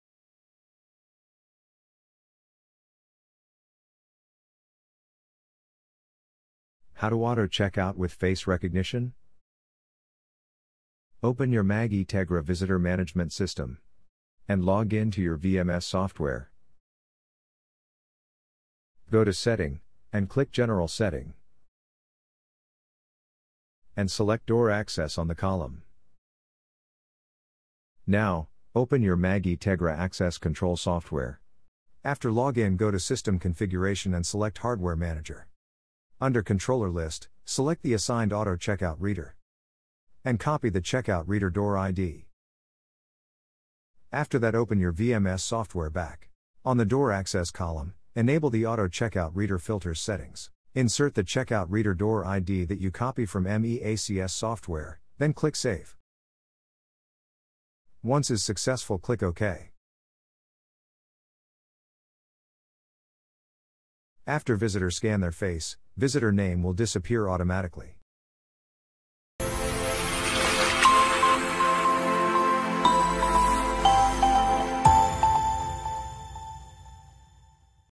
How to auto check out with face recognition using ME-VMS
This is video tutorial for client and end user that using our MagEtegra ME-VMS Visitor Management System software.